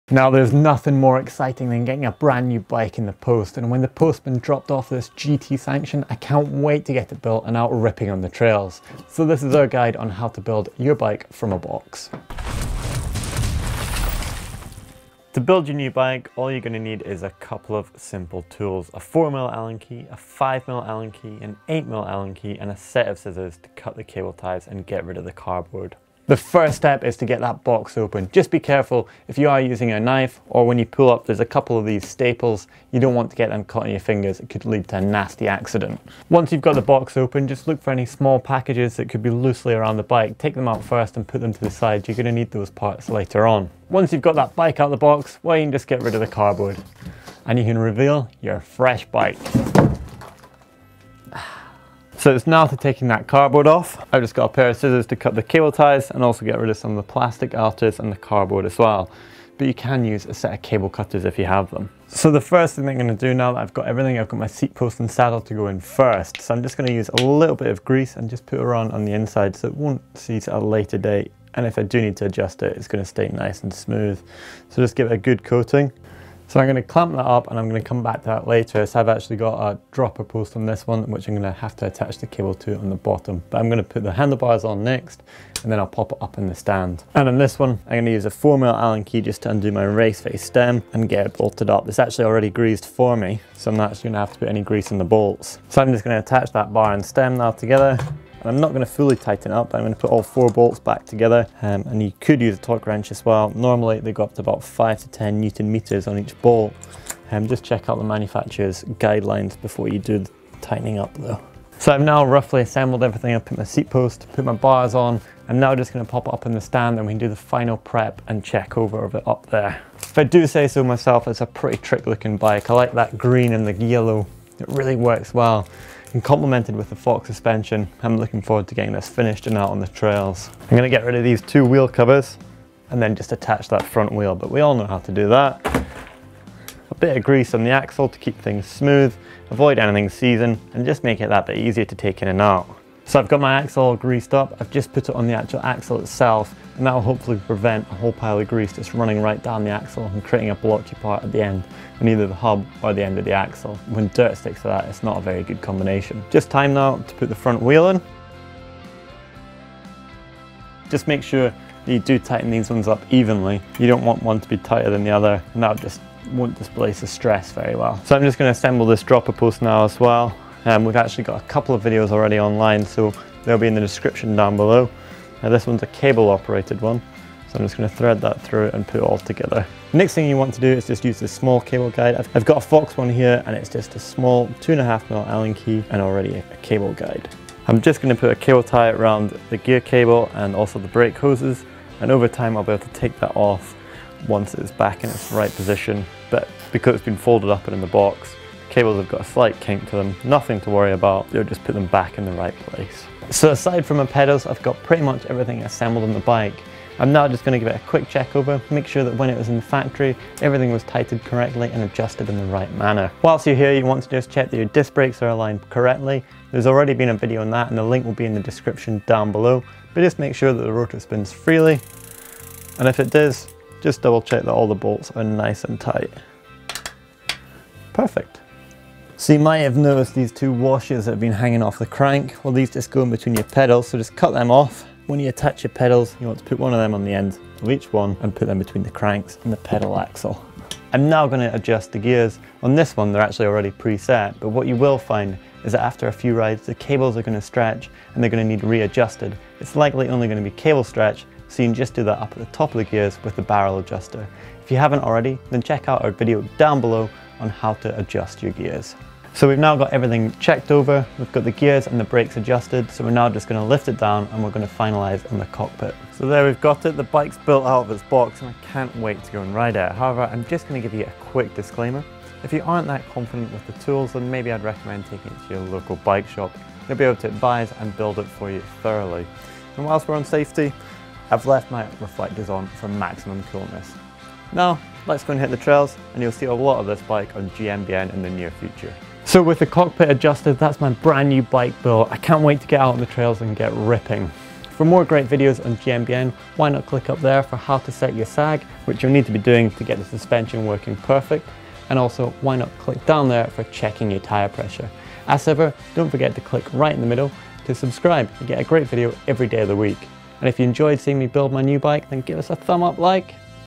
How To Build Your New Mountain Bike From The Box | Mountain Bike Maintenance
If you buy a bike online, it'll look something like this when it arrives.

Many mountain bikers choose to buy their bikes online. Most major internet retailers package their bikes up in a similar way the Scott's GT Sanction in this video. That is to say, it should be easy enough to build up, and you won't need too many tools. Check out the video and let us know how you get on!

Just one minor word of warning, if you're unsure about anything when you're building your bike up, head over to a bike shop and get them to build it up. It's a safety thing.